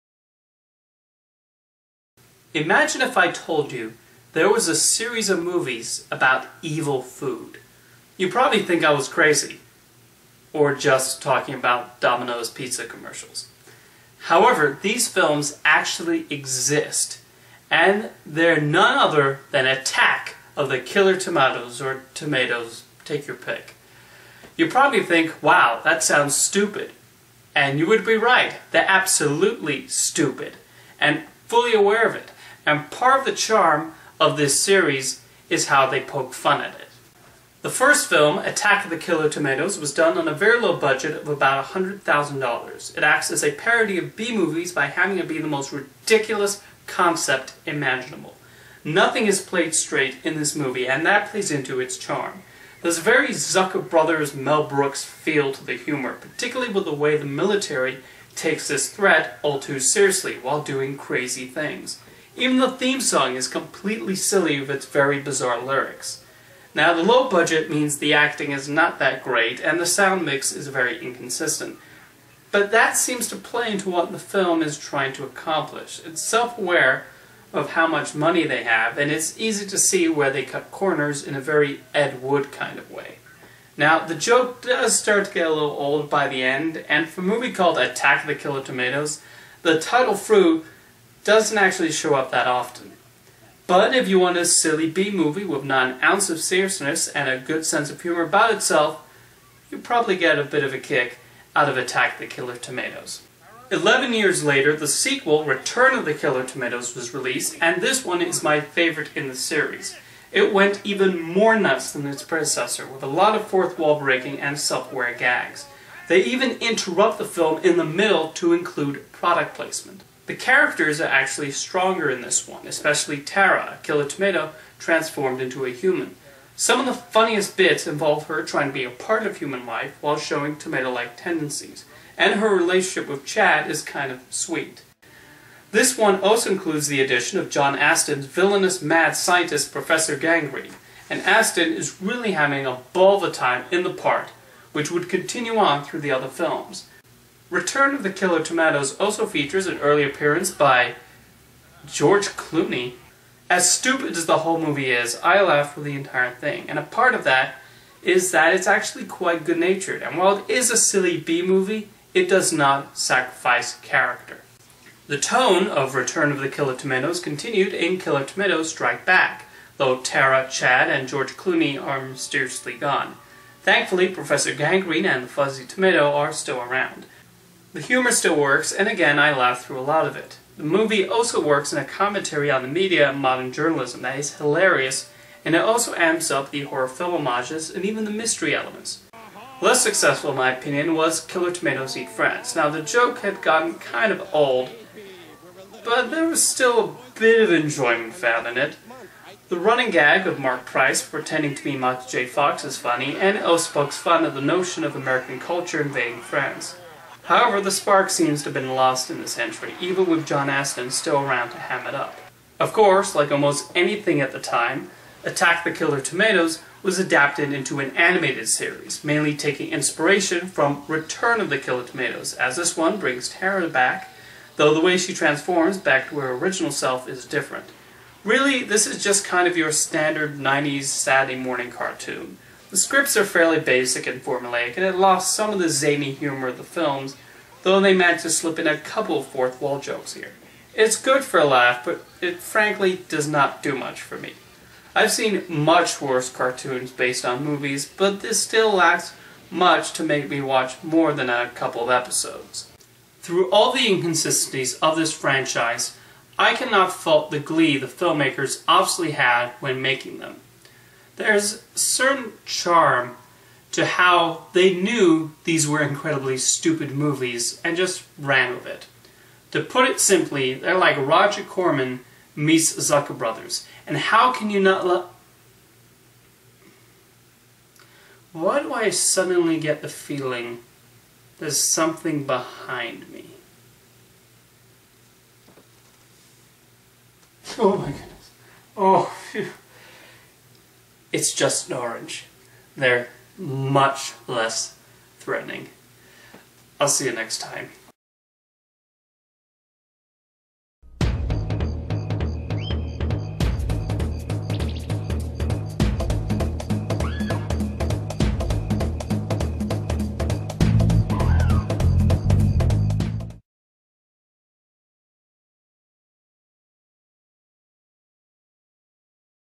A Look Back at "Attack of the Killer Tomatoes"
I talk about the B-movie parody series Attack of the Killer Tomatoes, which is completely and utterly stupid...and proud of it.